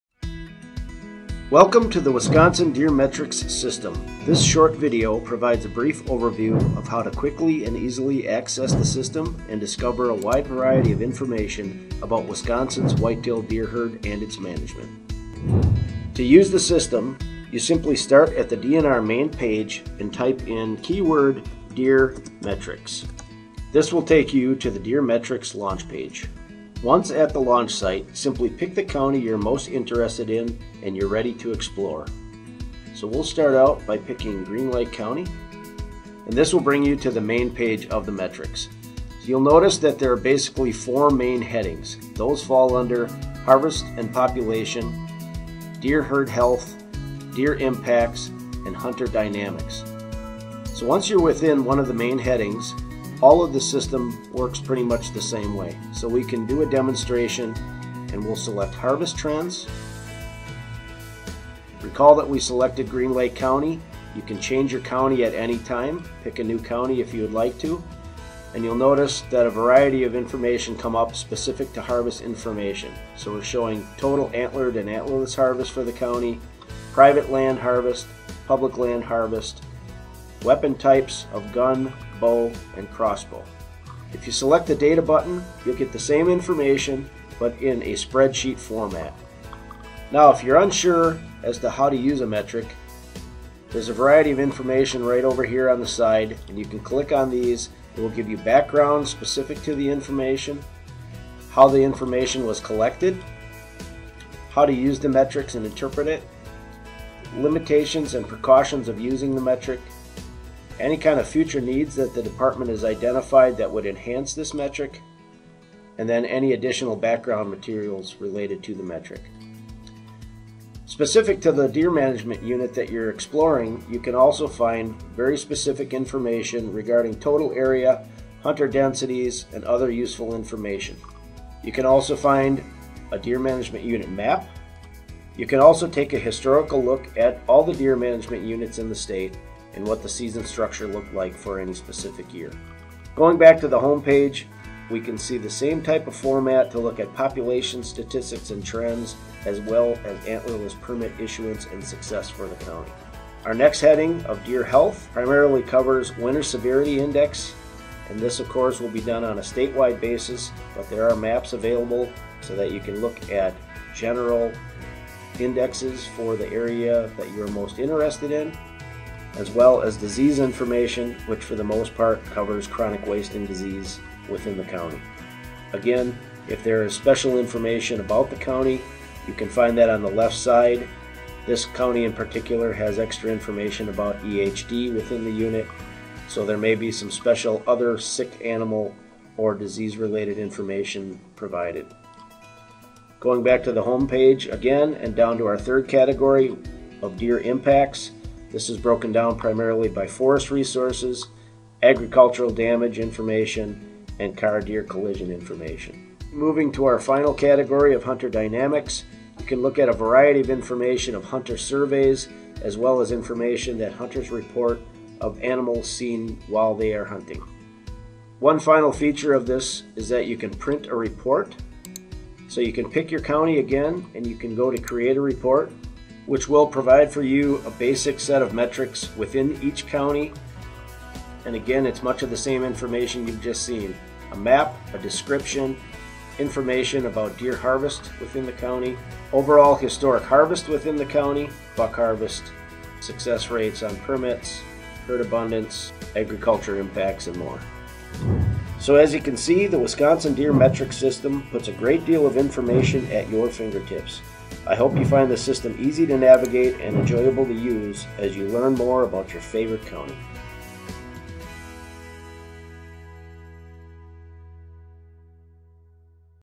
Deer Metrics Website Tutorial
Find all the stats and figures you've been looking for. The DNR's new Deer Metrics website puts the info at your fingertips. We hope this new tool helps hunters and conservationists everywhere.